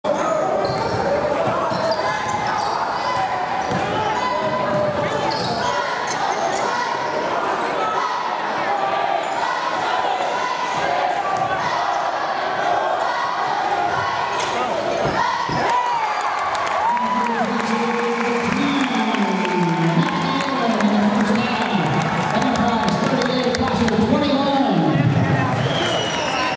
Boys 2012 Harlan Carter: Enterprise closes out first half with 3 ball.
Enterprise patiently works the clock down and Andrew Fitzhugh drills a 3 from the right corner to close out the half with a 38-21 lead.